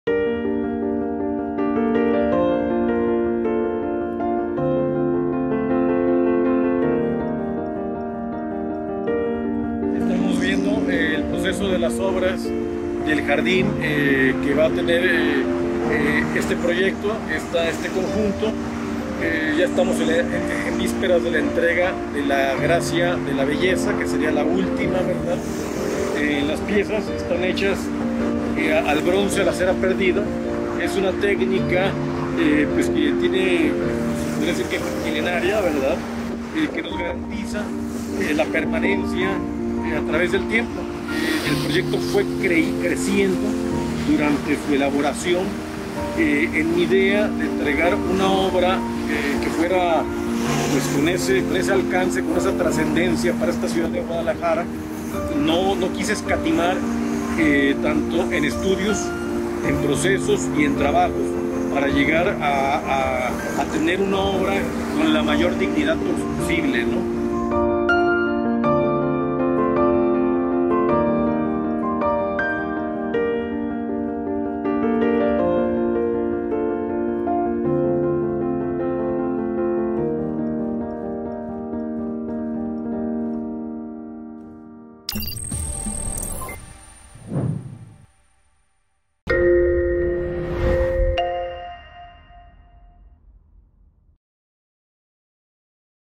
Avance de obras en las Tres Gracias de Sergio Garval
Estamos construyendo una plazoleta y un elevador que te permitirán ver de cerca “Las Tres Gracias”, las espectaculares esculturas del artista tapatío, Sergio Garval.

Las esculturas se encuentran en Lázaro Cárdenas casi en al cruce con Fuelle.

Este lugar será uno de los más apreciados por los locales y turistas que visiten Guadalajara.